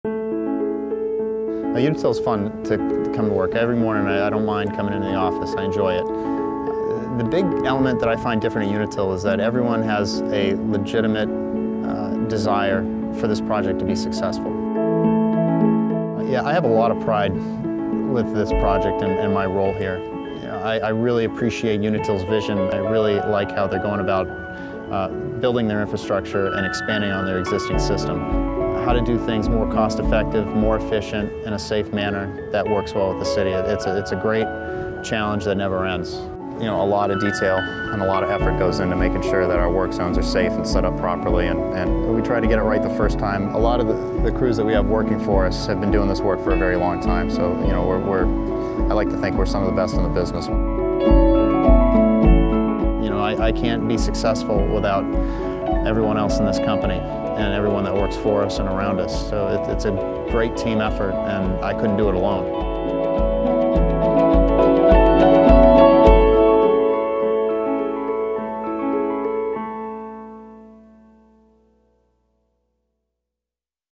Energy For Lifer - Matt Doughty
Enjoy this teaser and after watching, click here for the full version

Meet Matt Doughty, our S.U.R.E. cast iron replacement project manager in the greater Portland, ME area. Matt oversees this proactive, multi-year gas infrastructure project, coordinating with local residents and businesses to upgrade their service.